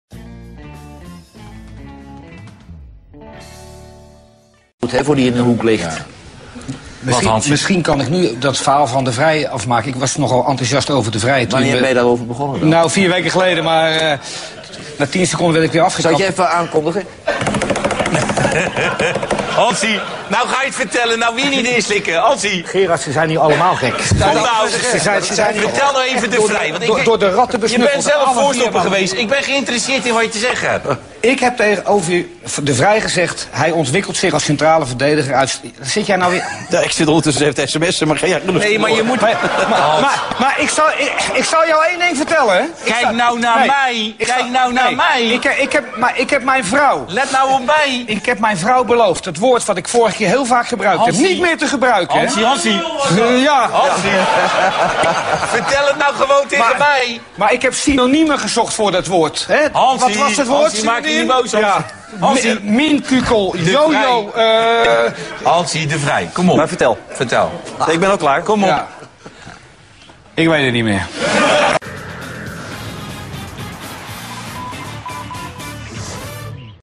Hans Kraay krijgt een fijne aankondiging door Johan Derksen
Johan Derksen zorgt voor een tromgeroffel als Hans Kraay het woord krijgt. Als hij het woord eenmaal heeft weet ie niet meer wat ie wilde zeggen.